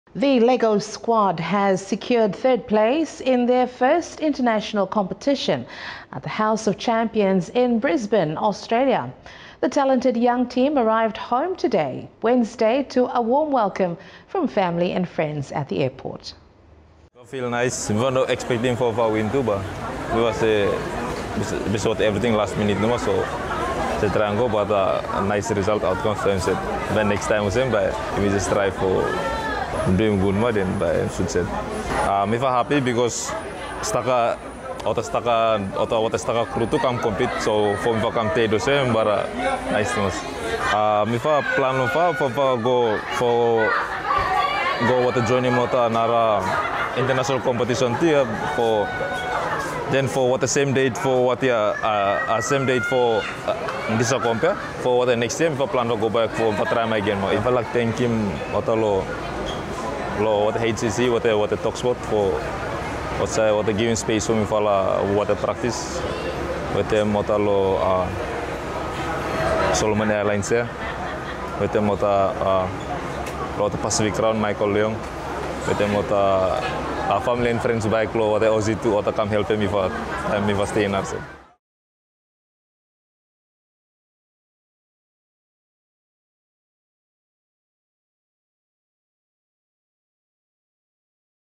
Lego Squad Welcomed Home After Securing Third in Brisbane!
The Lego Squad has secured third place in their first international competition at the House of Champions in Brisbane, Australia. The talented young team arrived home today Wednesday to a warm welcome from family and friends at the airport.